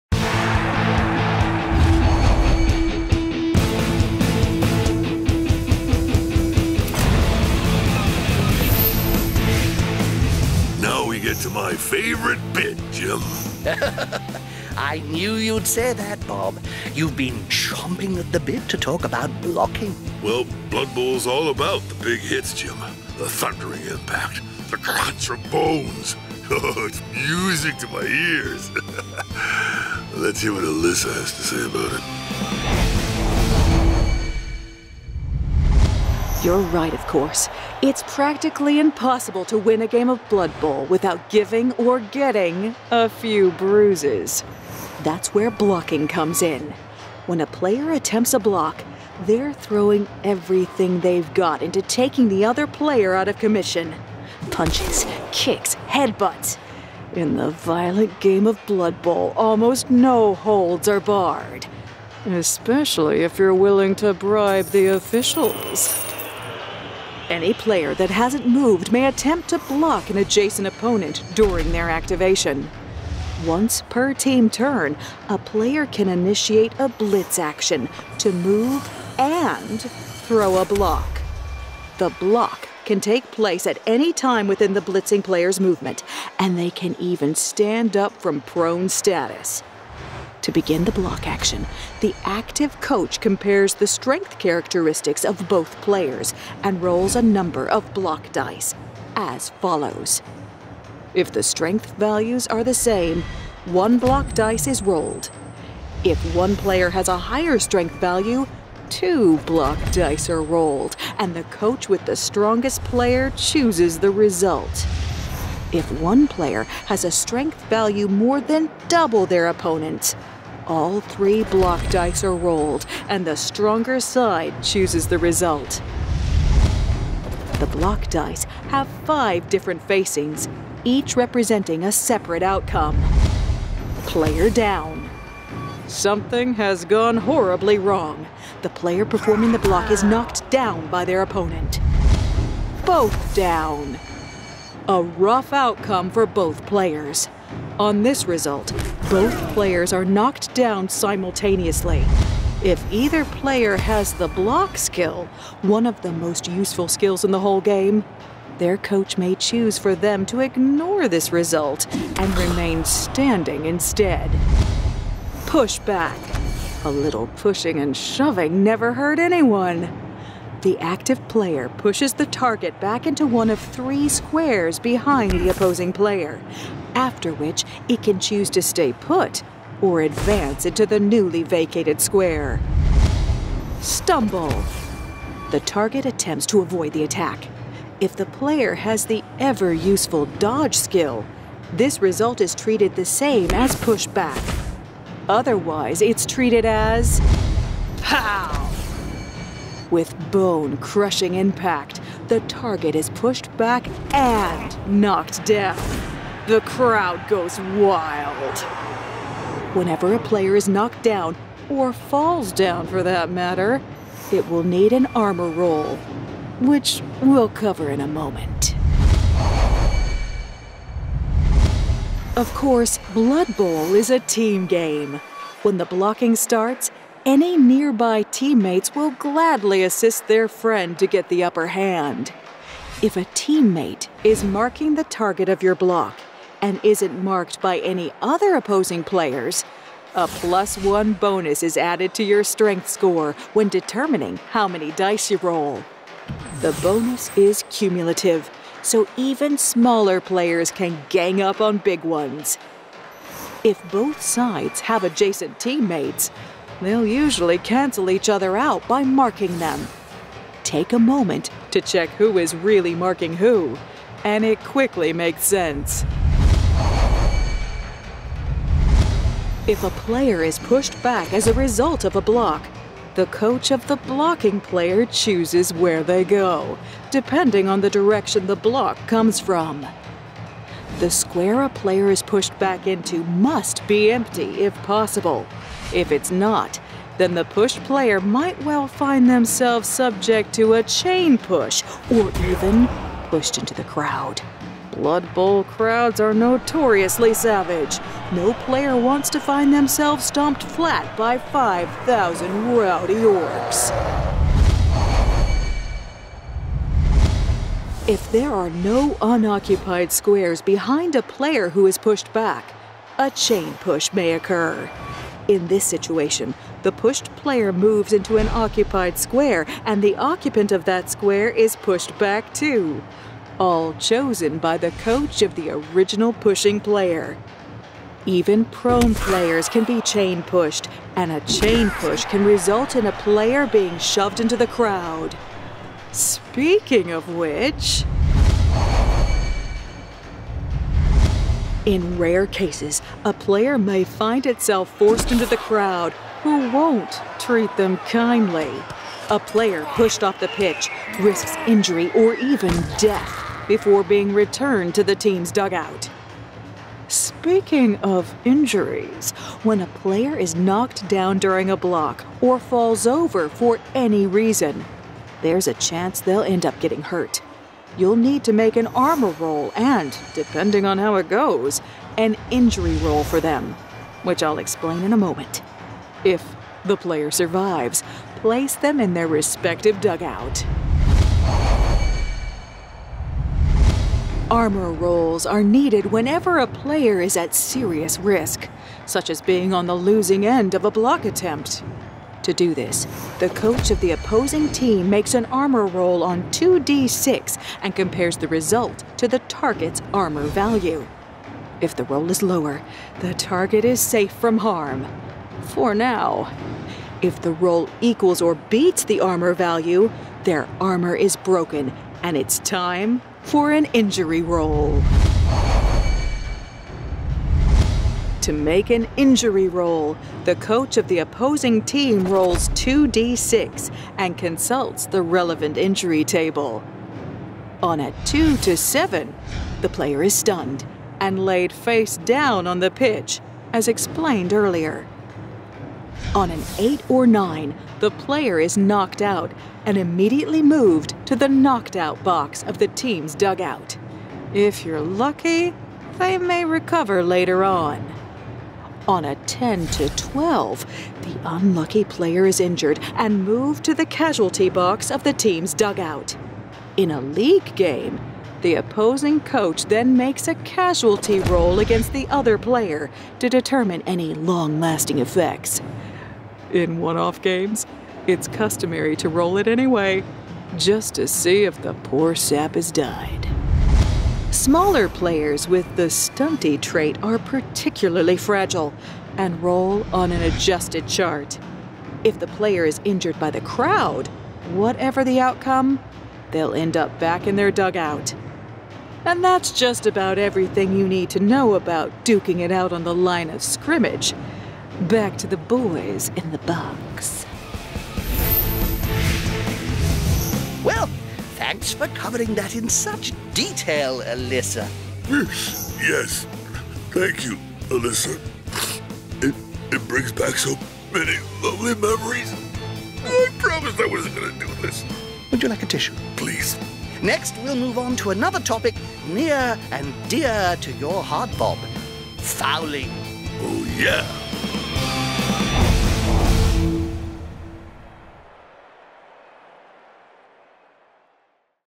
Learn to Play Blood Bowl – Blocking, Armour, and Injury
Bob: “Now this is the real heart of Blood Bowl. Is there anything more satisfying than vicious tackles, Jim?”
Jim: “I have to admit… not really. Let’s take a much closer look at how to dish out the hurt.”